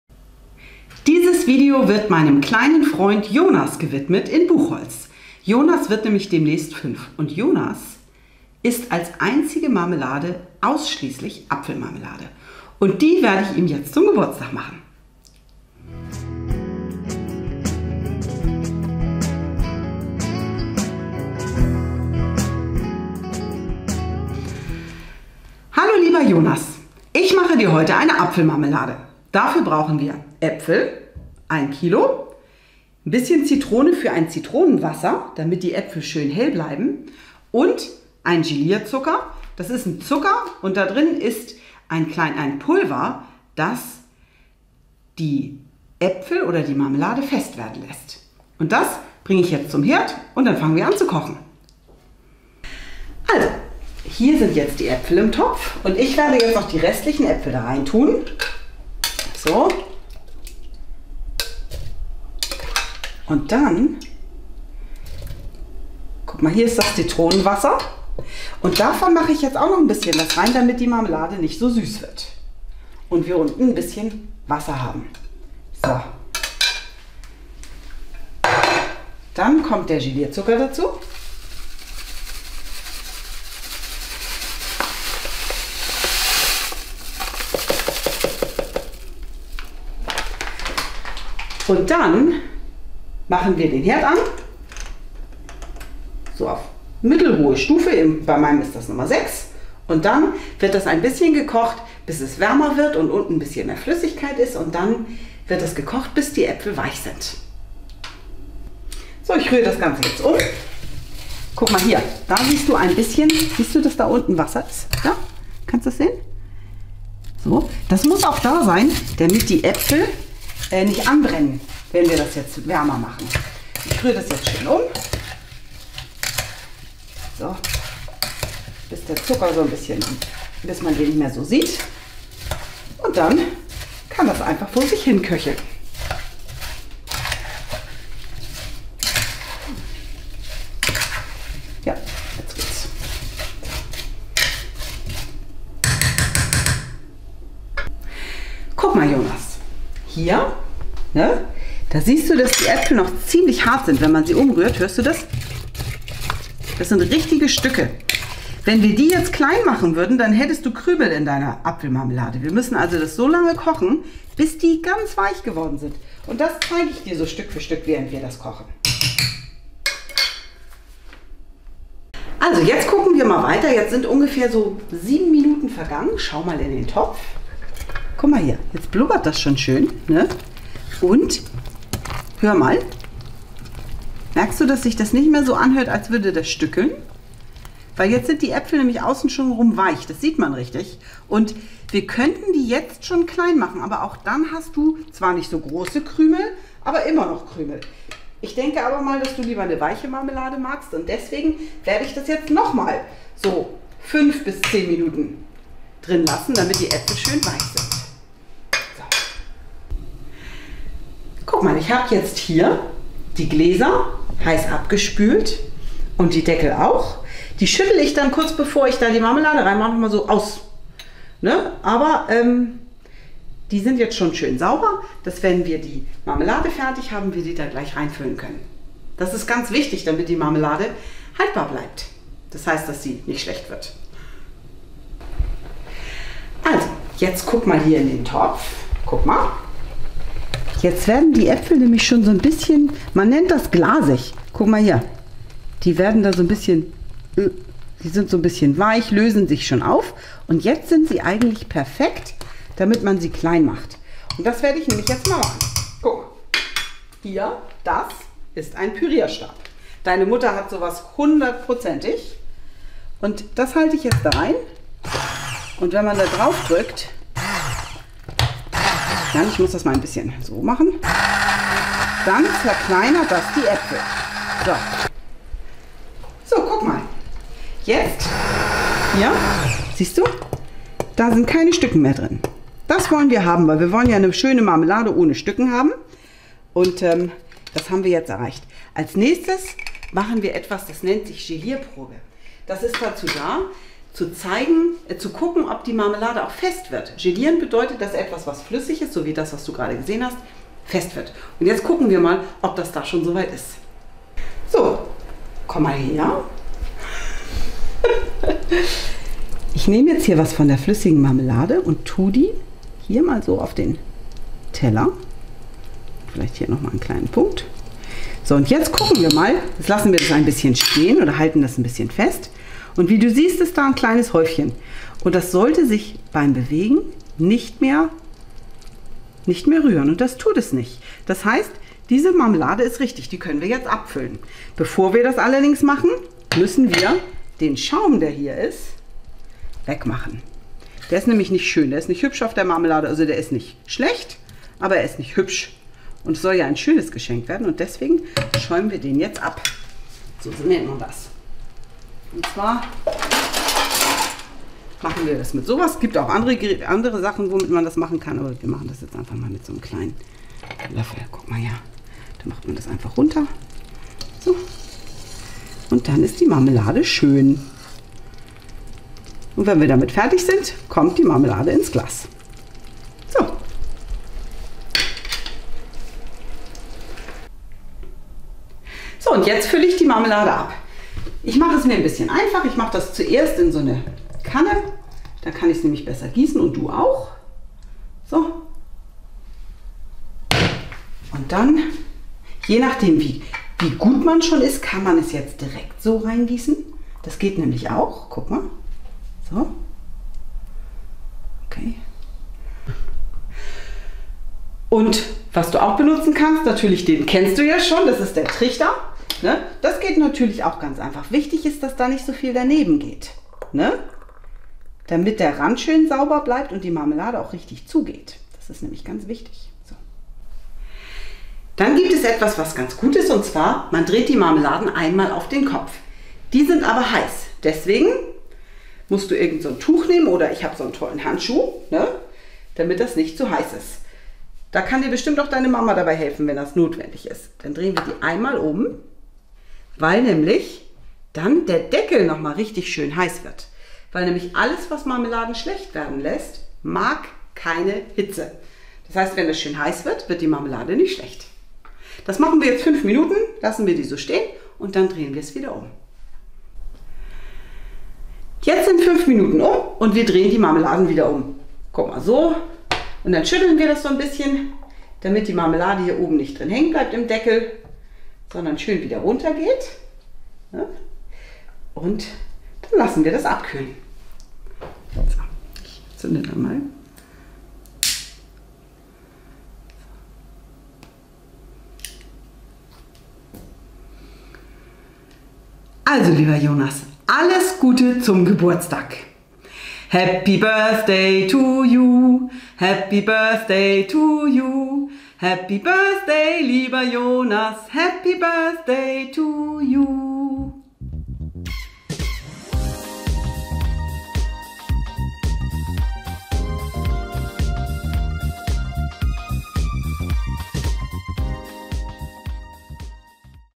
Geburtstagsgeschenk - Apfelmarmelade für Jonas
Mein kleiner Freund Jonas feiert seinen 5. Geburtstag und dafür werde ich ihm in diesem Video seine Lieblingsmarmelade kochen.
Mit einem kleinen Geburtstagsständchen am Ende :-)  (8:00)

Meine Erklärungen im Video richten sich speziell an Jonas, sind aber natürlich auch an alle Kinder gerichtet, die sich einmal am Einkochen von Marmelade versuchen wollen. Trotzdem gehe ich davon aus, dass wenn Kinder diese Apfemarmelade nachkochen, sie dies unter Aufsicht von Erwachsenen machen. Deshalb habe ich mir alle Hinweise, wo man aufpassen muss und wo es für Kinder gefährlich werden könnte, gespart.

Eine genauere Erklärung, was ein 2:1 Gelierzucker ist, welche anderen Gelierzuckerarten es gibt und welcher wann verwendet wird, findet ihr

Zutaten
1 kg Äpfel
Saft einer halben Zitrone und etwas Wasser
500 g 2:1 Gelierzucker

0:00 Einleitung
0:29 Die Zutaten: Das kommt rein in die Apfelmarmelade
0:53 Die Zubereitung: So wird die Marmelade gemacht
1:51 Tipp: So brennt das Obst beim Marmeladekochen nicht an
2:28 Die Äpfel sollten vor dem Pürieren weich gekocht werden
3:31 Die Gläser werden zum Abfüllen vorbereitet
4:48 Nach dem Pürieren der Früchte mache ich die Gelierprobe
5:34 Der Schaum wird entfernt
6:17 Die Apfelmarmelade wird heiß abgefüllt und schnell verschlossen
8:00 Die fertigen Gläser mit viel Deko und einem Ständchen